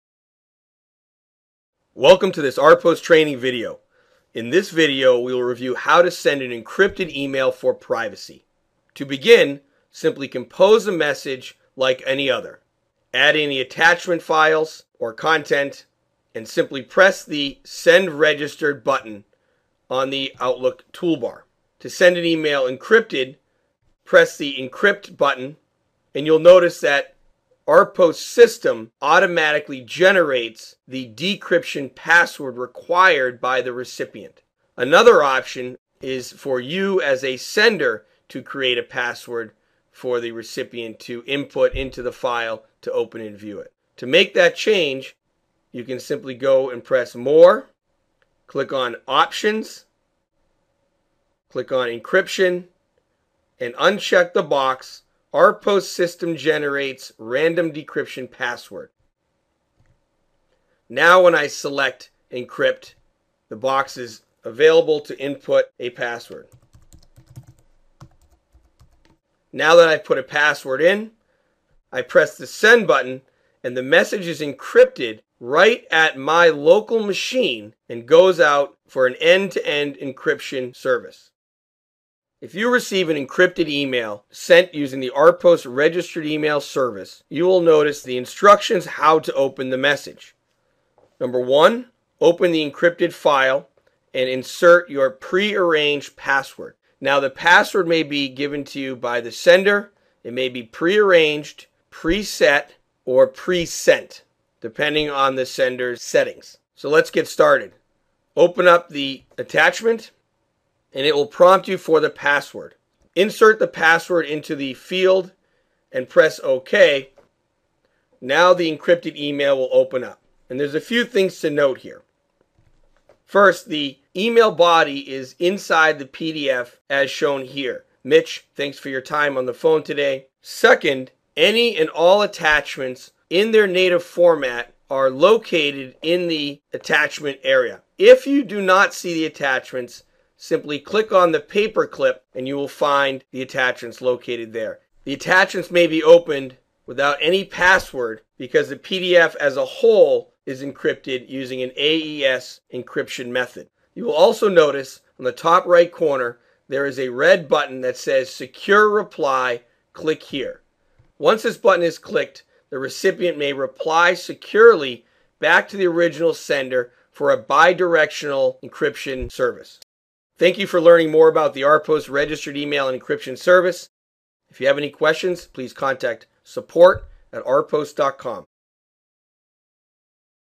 Email Encryption - How To Send & Receive Encrypted Email with RPost
This tutorial video demonstrates how to send an encrypted email using the RPost Registered Email Encryption service and how the recipient opens the email at their end.

Key Benefits of RPost's email encryption service:
• Encrypted messages delivered directly to recipient's inbox.
• Returns court- admissible proof of encryption and compliance.
• No third-party storage of messages or attachments at any time.
• Exceeds HIPAA, HI-TECH, FSA, GLB, SOX requirements.

As long as you have access to your email or a web browser you can send encrypted emails, eSign and send RPost Registered Email messages anywhere, anytime as your all-in-one, Total Email Service!